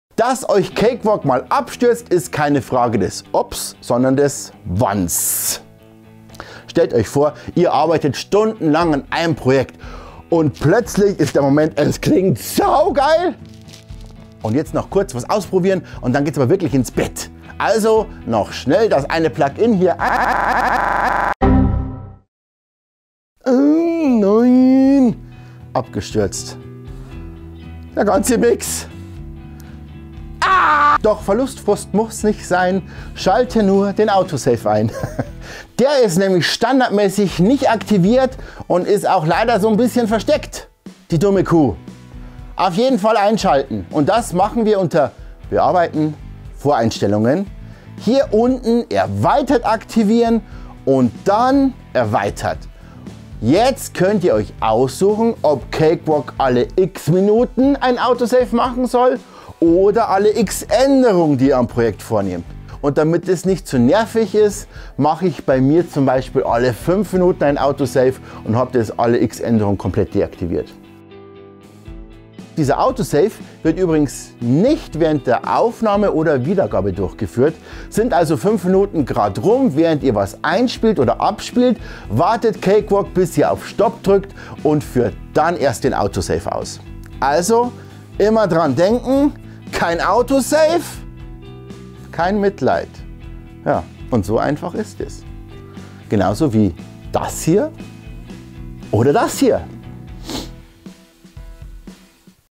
Cakewalk Autosave: Vermeide Datenverlust mit diesen einfachen Schritten
Schütze deine Arbeit in Cakewalk by Bandlab vor Datenverlust!
In diesem Video zeige ich dir, wie du den Autosave aktivierst und dein Projekt automatisch speichern lassen kannst. Der Cakewalk Autosave ist standardmäßig deaktiviert, aber mit nur wenigen Klicks kannst du dein Projekt in vorgegebenen Abständen automatisch sichern. Vermeide Datenverlust und aktiviere jetzt den Autosave in Cakewalk! Dauert keine 2 Minuten.

Auch ist Cakewalk eine kostenlose DAW, die mindestens mal das gleiche kann, wie z.B. Avid ProTools. Einfach mal eine kostenlose Alternative zu Cubase, Logic und anderen Mitbewerbern.

Inhalt
0:00 Cakewalk stürzt ab
0:30 Autosave einschalten
0:53 Speicherintervall einstellen
1:12 Kein Autosave bei Aufnahme / Wiedergabe